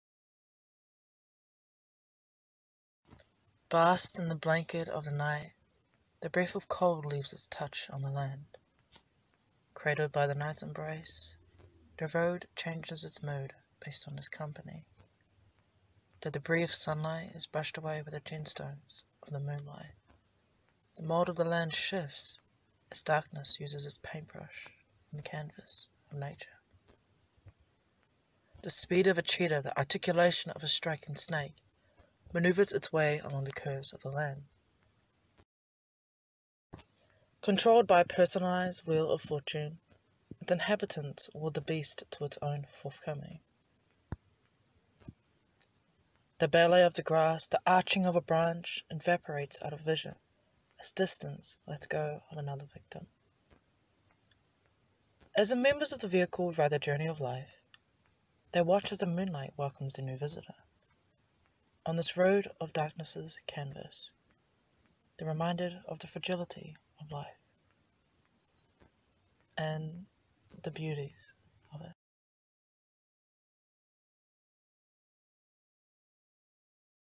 Darkness' Paintbrush (Audio), By Marcia Norman
This is the spoken verbal version of my poem. The only difference being this being a read-out version of the official poem.

This is the story about the journey through the unknown and compromising situation of a recovery of conflict. Poems are open to interpretation and personal identification and I love that.

Darkness' Paintbrush, By Marcia Norman
Poem/Video written and edited by Marcia Norman.

This poem is copyrighted and protected under law. If you wish to share it , it must be approved by me (the poet and editor).

Marcia Norman